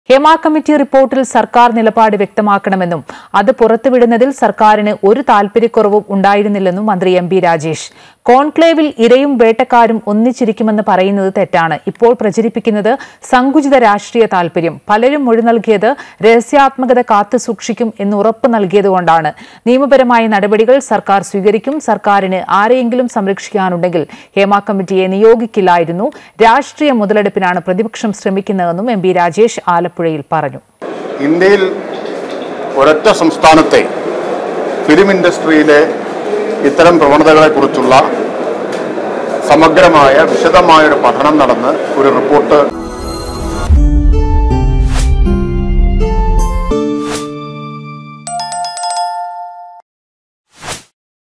'റിപ്പോര്‍ട്ട് പുറത്തുവിടാൻ സർക്കാരിന് താത്പര്യക്കുറവുണ്ടായിരുന്നില്ല' ; എം ബി രാജേഷ് | Amrita News
ഹേമ  കമ്മിറ്റി റിപ്പോർട്ടിൽ സർക്കാർ നിലപാട് വ്യക്തമാണെന്നും അത്
പുറത്തുവിടുന്നതിൽ സർക്കാരിന് ഒരു താൽപര്യക്കുറവും ഉണ്ടായിരുന്നില്ലന്നും മന്ത്രി എം ബി  രാജേഷ്. കോൺക്ലേവിൽ ഇരയും വേട്ടക്കാരും ഒന്നിച്ചിരിക്കുമെന്ന് പറയുന്നത് തെറ്റാണ്. ഇപ്പോൾ പ്രചരിപ്പിക്കുന്നത് സങ്കുചിത രാഷ്ട്രീയ താൽപര്യം. പലരും മൊഴി നൽകിയത് രഹസ്യാത്മകത കാത്ത് സൂക്ഷിക്കും എന്നു ഉറപ്പ് നൽകിയതു കൊണ്ടാണ്.